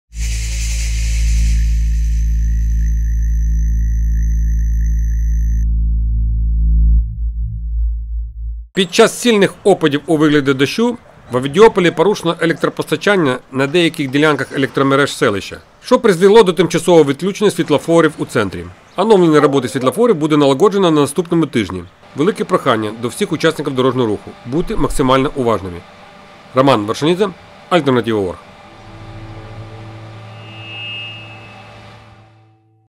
В ОВІДІОПОЛІ ТИМЧАСОВО НЕ ПРАЦЮЮТЬ СВІТЛОФОРИ
Під час сильних опадів у вигляді дощу, в Овідіополі порушено електропостачання на деяких ділянках електромереж селища, що призвело до тимчасового відключення світлофорів у центрі селища.
Оновлення роботи світлофорів буде налагоджено на наступному тижні. Велике прохання до всіх учасників дорожнього руху — бути максимально уважними!
По матеріалах: Овідіопольська селищна рада
Роман Варшанідзе
АЛЬТЕРНАТИВА.ОРГ